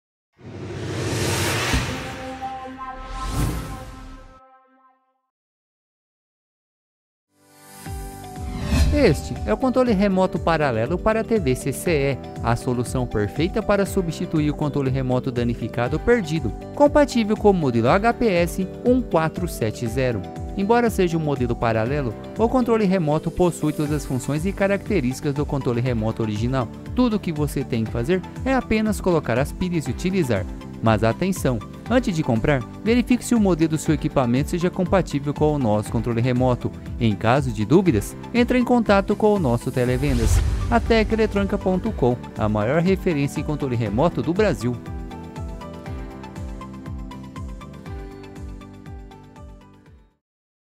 Controle Remoto CCE C0214B (780015)
Controle Remoto para TV CCE

Modelos Compatíveis: HPS1470 / HPS1490 / HPS2070 / HPS2090

Embalagem: 01 Unidade

Garantia: 90 dias

Fabricante: Produto Importado